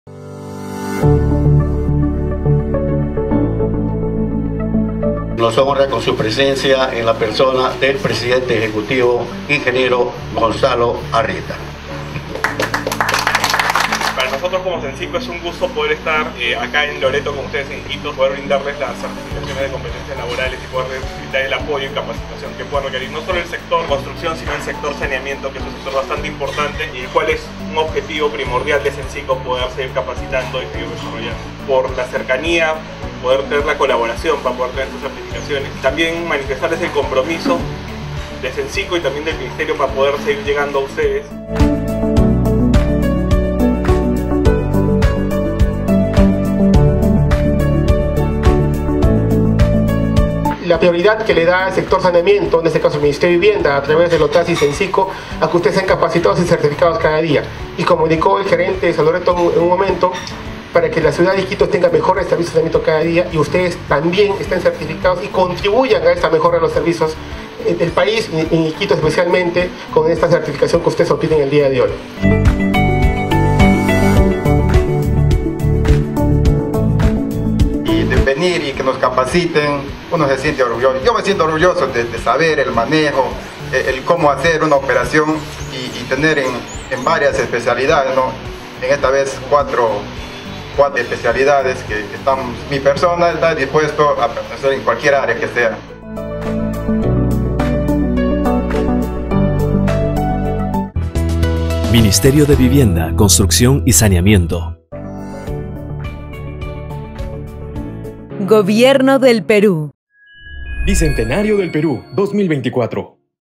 Ceremonia de Certificación de Copetencias Laborales en el sector Saneamiento en la EPS Seda Loreto.
Desde la ciudad de Iquitos el SENCICO continua certificando las competencias laborales de más peruanos y peruanas. Esta vez, certificamos a 31 trabajadores de la EPS Sedaloreto S.A. gracias al convenio con OTASS 💧

✅ En la ceremonia participaron el Presidente Ejecutivo del SENCICO, Ing. Gonzalo Arrieta, el Director Ejecutivo de la OTASS, Ing. Héctor Barreda, el Gerente Zonal de Iquitos, CPC. Marcos Augusto Rodríguez Chu y el Gerente General de EPS SEDALORETO, Lic. José Luis García Cardich.

➡️Durante la ceremonia el presidente ejecutivo del SENCICO ratificó el objetivo que se tiene como institución para seguir capacitando a más trabajadores a nivel nacional, mencionó que "Para nosotros como SENCICO es un gusto estar en Iquitos y poder brindarles la certificación de competencias laborales, así como, el apoyo y capacitación que puedan requerir, no solo en el sector construcción sino en saneamiento, ya que es un sector bastante importante, y el cual es un objetivo primordial para poder seguir capacitando y desarrollando".👷‍♂️👷‍♀️